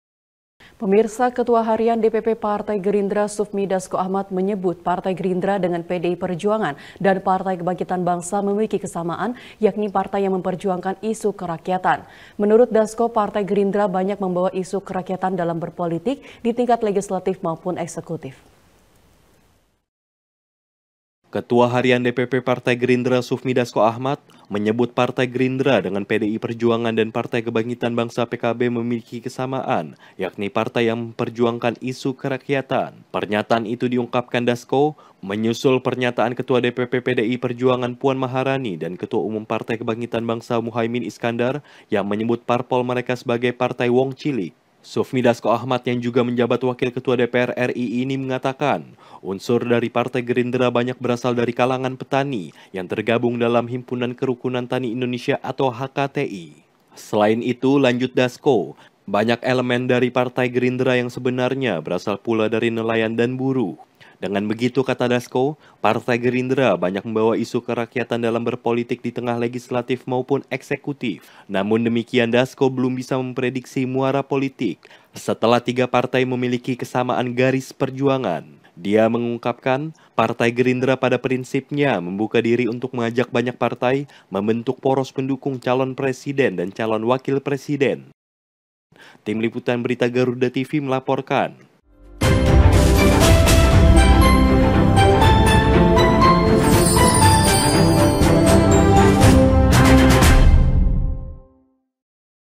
Partai Gerindra Punya Kesamaan Dengan PDIP dan PKB
Ketua Harian DPP Partai Gerindra Sufmi Dasco Ahmad menyebut Partai Gerindra dengan PDI Perjuangan dan Partai Kebangkitan Bangsa memiliki kesamaan yakni partai yang memperjuangkan isu kerakyatan. Menurut Dasco, Partai Gerindra banyak membawa isu kerakyatan dalam berpolitik di tingkat legislatif maupun eksekutif.